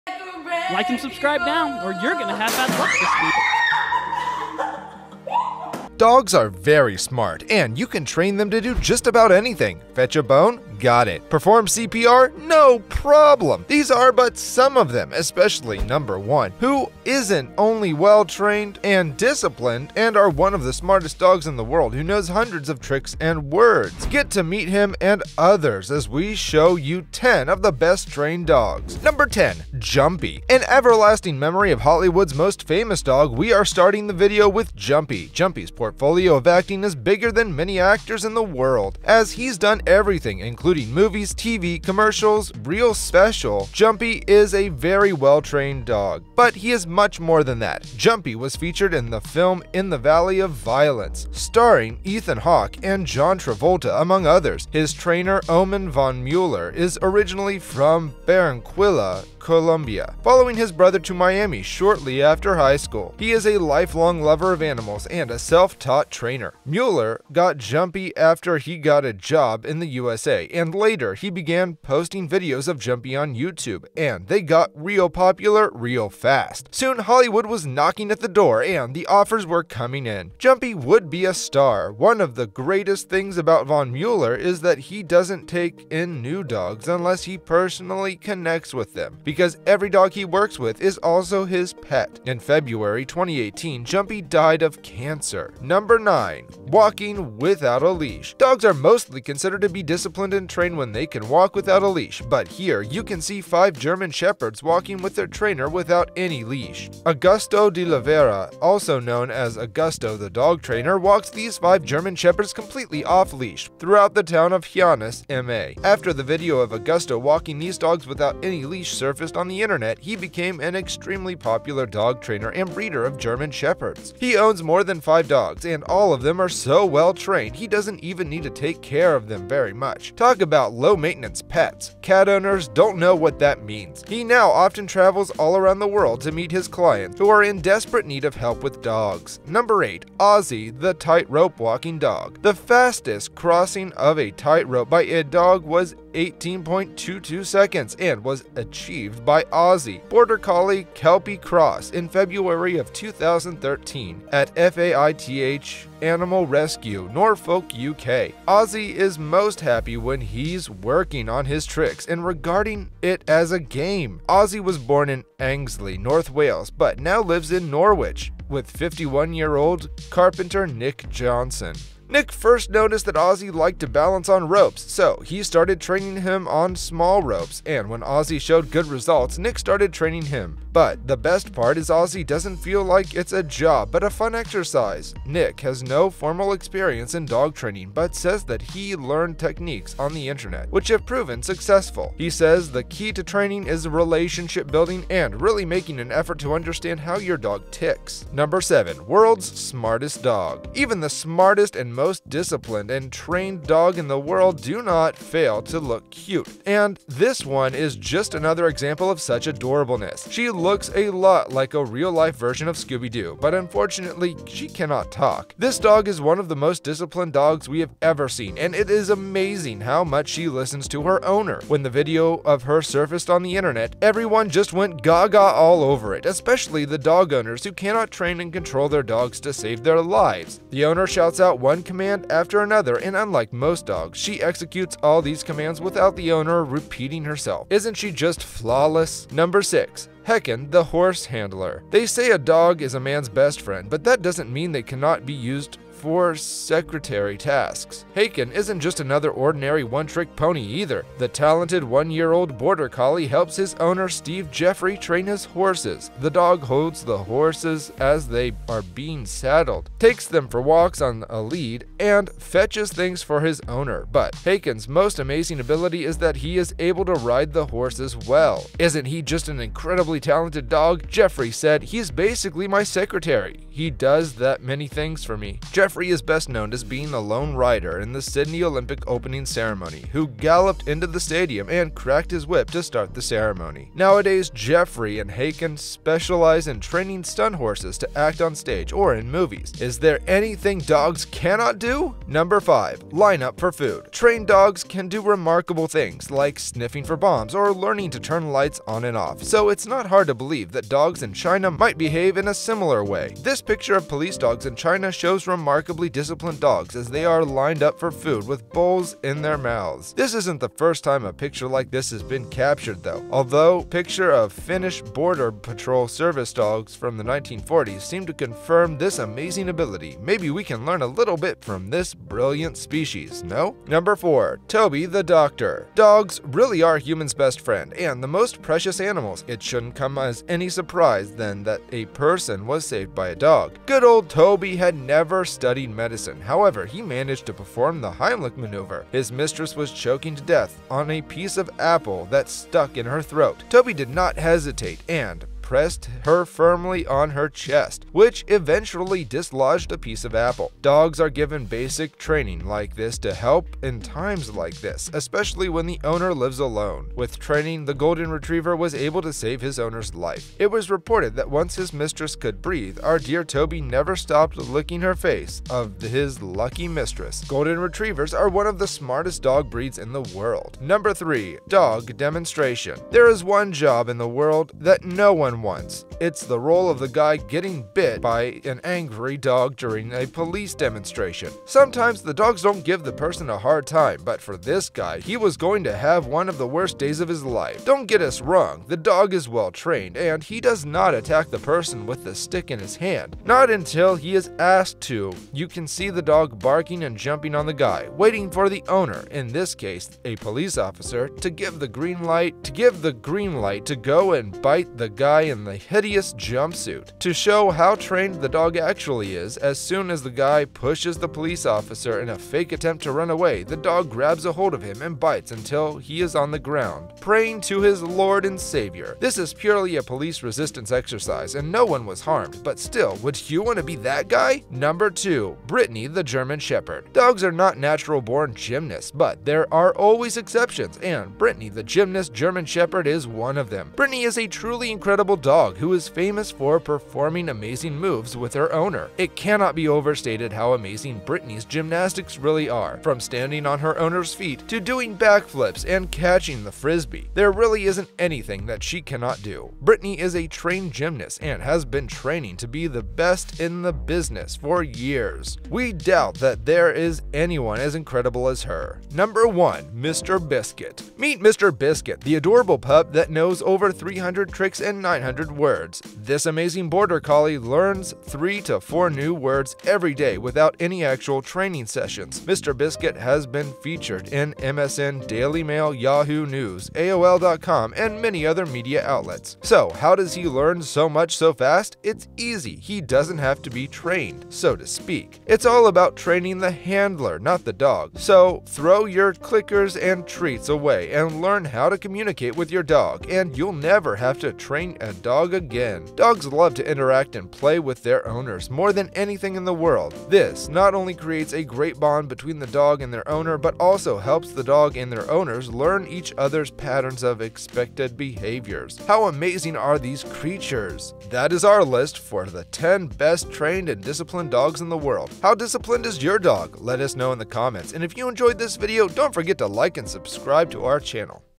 10 Best Trained & Disciplined Dogs in The World!
10 Best Trained & Disciplined Dogs in The World!

Dogs are very smart, and you can train them to do just about anything. Fetch a bone? Got it. Perform C.P.R.? No problem. These are but some of them, especially number one, who isn't only well-trained and disciplined and one of the smartest dogs in the world who knows hundreds of tricks and words. Get to meet him and others as we show you 10 of the best-trained dogs!
Number 10. Jumpy
In everlasting memory of Hollywood's most famous dog, we are starting the video with Jumpy. Jumpy's portfolio of acting is bigger than many actors in the world as he's done everything, including Movies, T.V., Commercials. Real special. Jumpy is a very well-trained dog, but he is much more than that. Jumpy was featured in the film "In The Valley Of Violence," starring Ethan Hawke and John Travolta, among others. His trainer, Oman von Muller, is originally from Barranquilla, Colombia, following his brother to Miami shortly after high school. He is a life-long lover of animals and a self-taught trainer.
Muller got jumpy after he got a job in the U.S.A., and later, he began posting videos of Jumpy on Youtube, and they got real popular real fast. Soon, Hollywood was knocking at the door, and the offers were coming in. Jumpy would be a star. One of the greatest things about von Muller is that he doesn't take in new dogs unless he personally connects with them. Because every dog he works with is also his pet. In February 2018, jumpy died of cancer
Number 9.) Walking without a leash
Dogs are mostly considered to be disciplined and trained when they can talk without a leash. But here, you can see five German shepherds walking with their trainer without any leash. Augusto Deoliveira, also known as Augusto the Dog Trainer, walks these 5 German Shepherds completely off-leash throughout the town of Hyannis, MA. After the video of Augusto walking these dogs without any leash surfaced on the internet, he became an extremely popular dog trainer and breeder of German Shepherds. He owns more than five dogs, and all of them are so well trained, he doesn't even need to take care of them much. Talk about low-maintenance pets. Cat owers don't know what that means. He now often travels all around the world to meet his clients who are in desperate need of help with his dogs.
Number 8. Ozzy The Tightrope Walking Dog
The fastest crossing of a tightrope by a dog was 18.22 seconds and was achieved by Ozzy, Border Collie/Kelpie cross, in February of 2013 at F.A.I.T.H. Animal Rescue, Norfolk, UK. Ozzy is most happy when he's working on his tricks and regards it as a game. Ozzy was born in Anglesey, North Wales, but now lives in Norwich with 51-year-old Carpenter Nick Johnson. Nick first noticed that Ozzy liked to balance on ropes. So, he started training him on small ropes, and when Ozzy showed good results, Nick started training him. But the best part is Ozzy doesn't feel like it's a job but a fun exercise. Nick has no formal experience in dog training but says that he learned techniques on the internet which have proven successful. He says the key to training is "relationship building and really making an effort to understand how your dog ticks".
Number 7) World's smartest dog
Even the smartest and the most disciplined and trained dogs in the world do not fail to look cute, and this one is just another example of such adorableness. She looks a lot like the real-life version of scooby doo, but unfortunately, she cannot talk. This dog is one of the most disciplined dogs we have ever seen, and it is amazing how much she listens to her owner. When the video of her surfaced on the internet, every just went gaga all over it, especially the dog owners who cannot train and control their dogs to save their lives. The owner shouts out one command after another, and unlike most dogs, she executes all those commands without the owner repeating himself. Isn't she just flawlessly?

Number 6. Hekan, the horse handler
They say a dog is a man's best friend, but that doesn't mean they cannot be used for secretary tasks. Hekan isn't just another ordinary one-trick pony either. The talented one-year-old border collie helps his owner Steve Jeffery